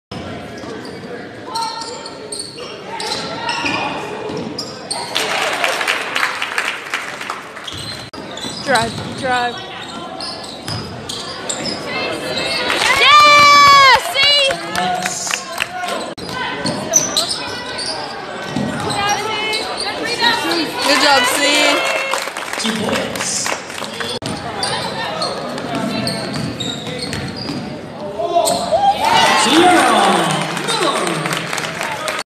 #31 Cierra Miller's Made Field Goals 2019-2020
Dover Eagles Varsity Girls Basketball
#31 Cierra Miller's made field goals during the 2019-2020 season